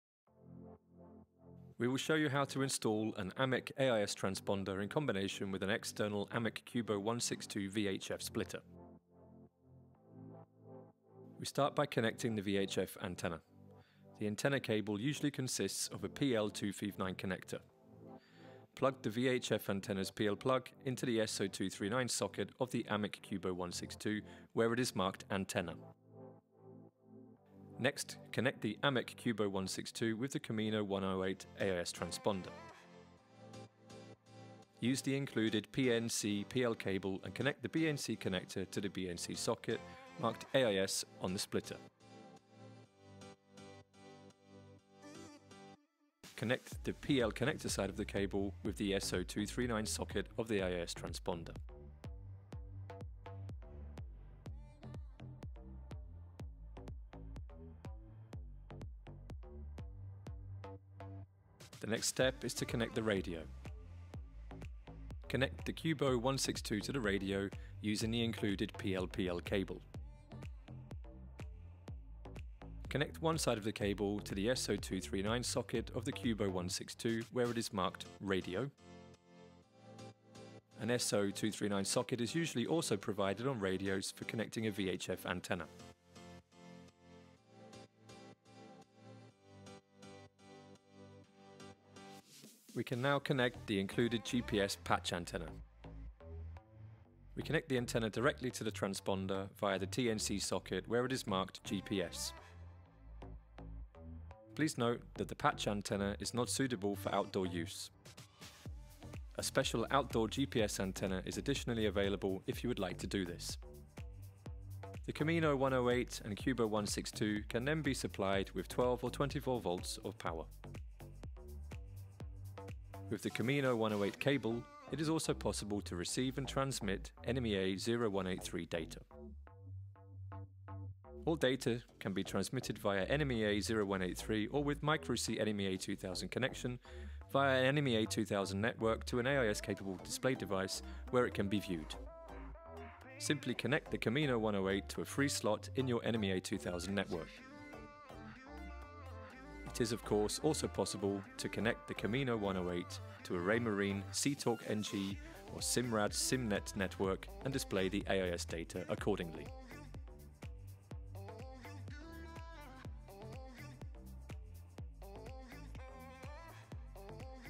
AIS Transponder - Installation with External Splitter | SVB
In this video we will show you how to install an AMEC Camino 108 AIS transponder in connection with an external AMEC Cubo 162 VHF antenna splitter.

📌 Short description:
An external antenna splitter means that a second VHF antenna is not required. Use a VHF antenna for simultaneous reception and transmission of AIS signals and radio messages.